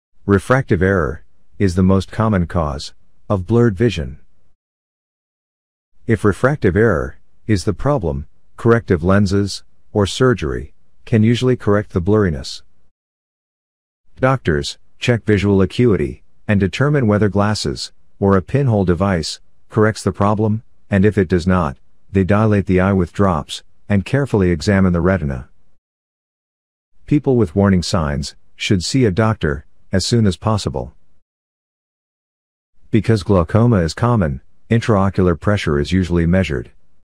Key Points of Blurred vision
1,  Refractive error is the most common cause of blurred vision.
2,  If refractive error is the problem, corrective lenses or surgery (particularly if cataracts are the cause of the change in refractive error) can usually correct the blurriness.
3, Doctors check visual acuity and determine whether glasses or a pinhole device corrects the problem, and if it does not, they dilate the eye with drops and carefully examine the retina.
4, People with warning signs should see a doctor as soon as possible.
Because glaucoma is common, intraocular pressure is usually measured.